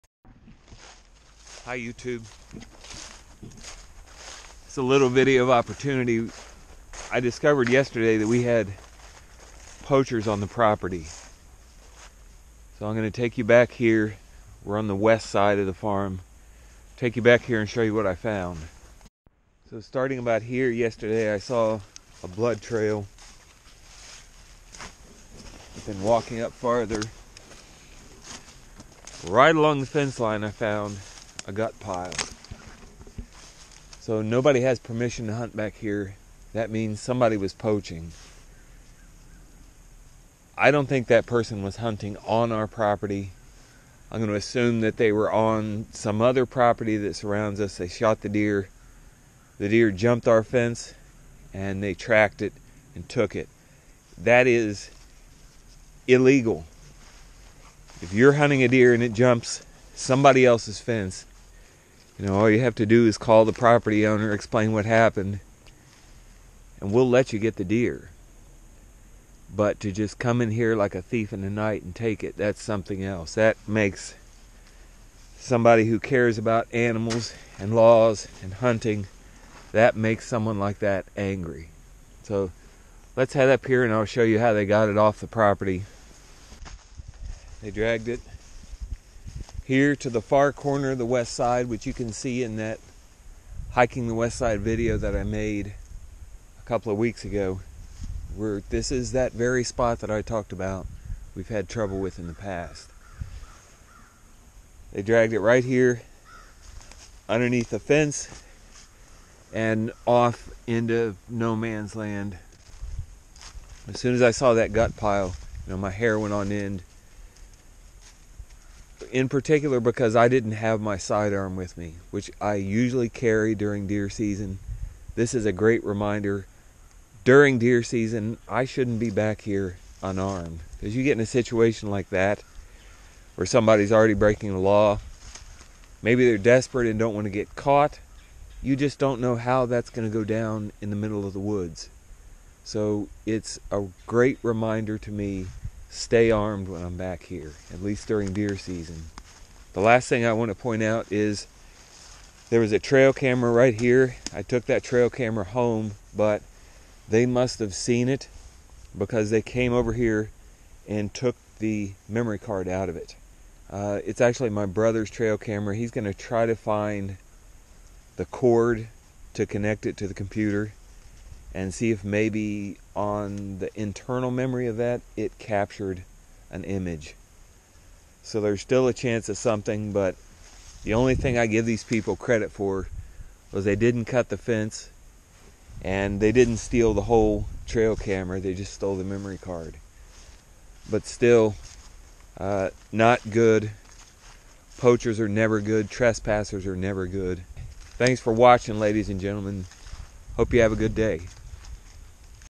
deer poachers on the farm!!
We found evidence of illegal poaching on the farm. The incident was reported to ODNR.